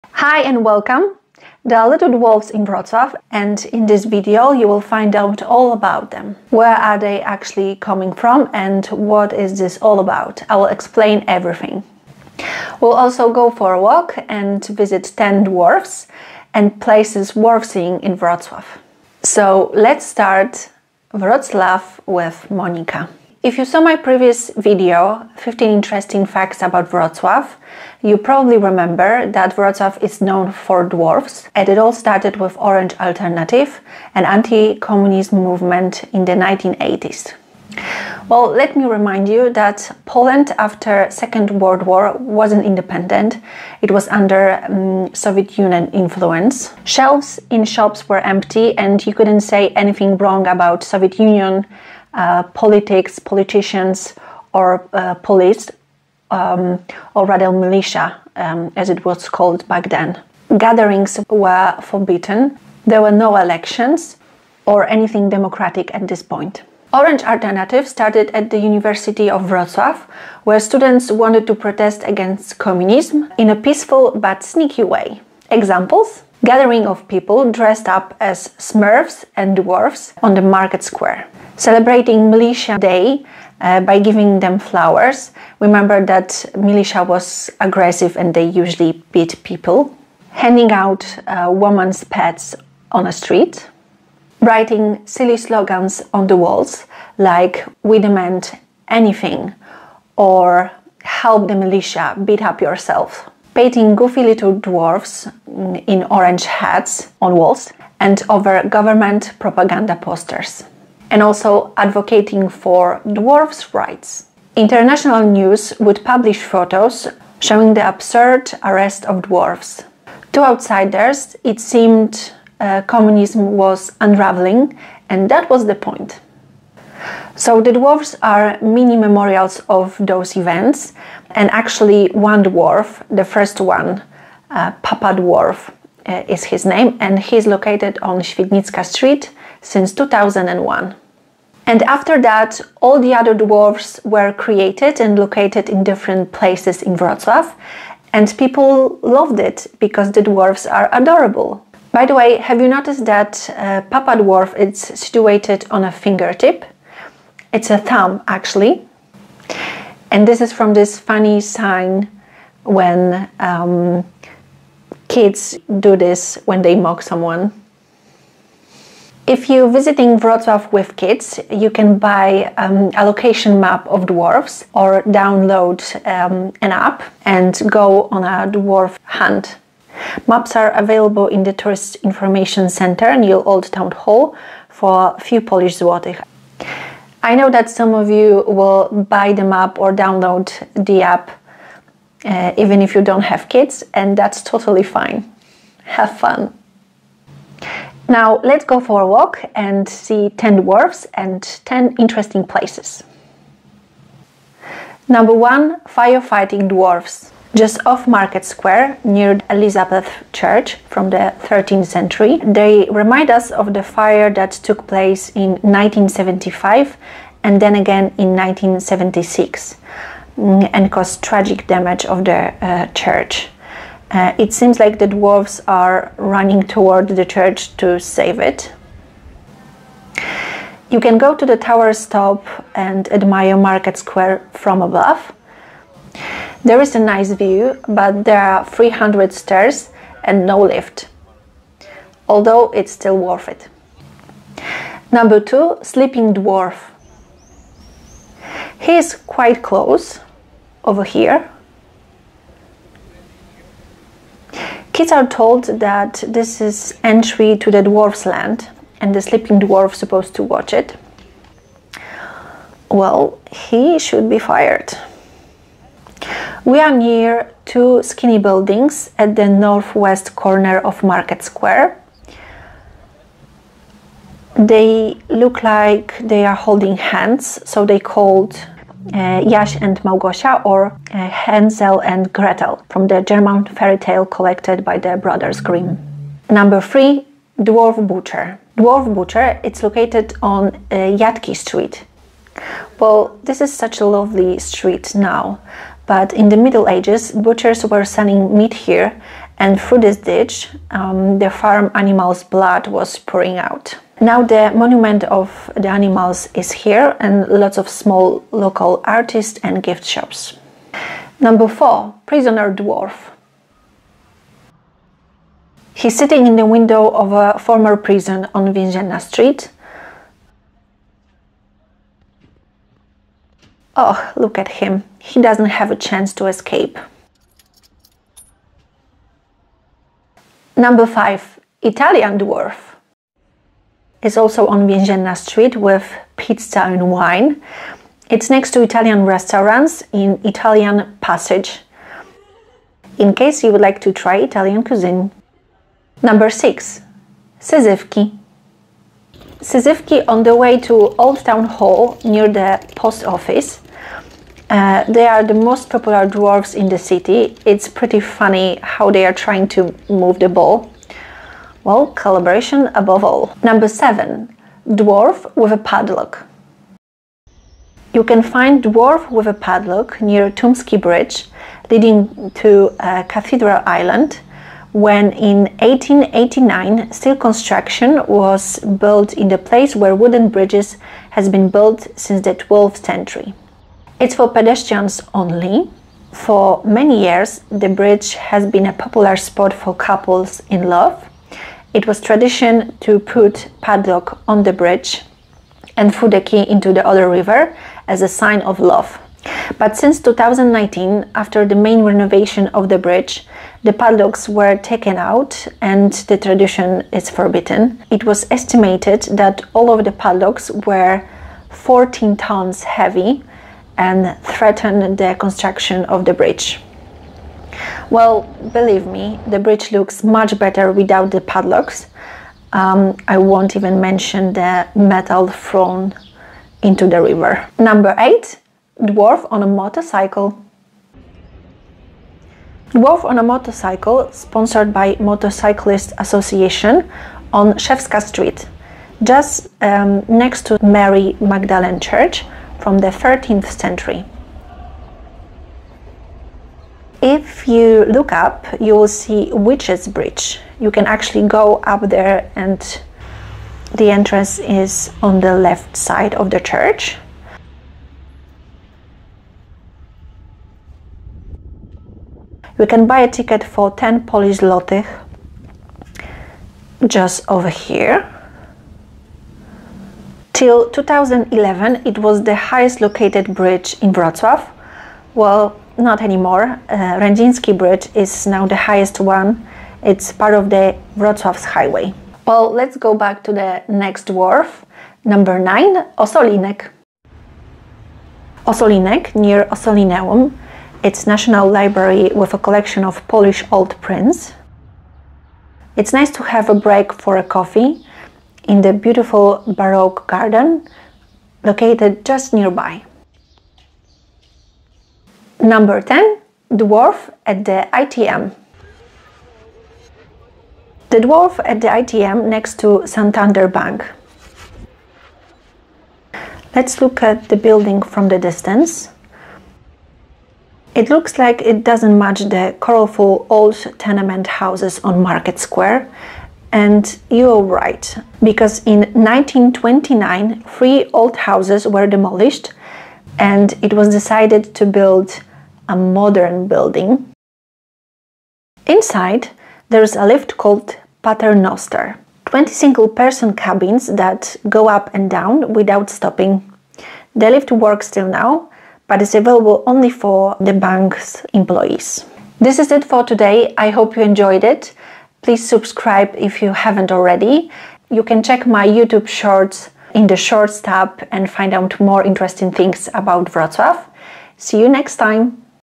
Dwarfs in Wroclaw
Where are dwarfs in Wroclaw actually coming from and what is this all about? Let's check their history and go for a walk to visit 10 of the 300 dwarfs we have in Wroclaw.

Do you plan to visit Wroclaw during your vacations or business trip and would like to know more about it? Or do you already live here or nearby and would like to learn more about the places you see every day?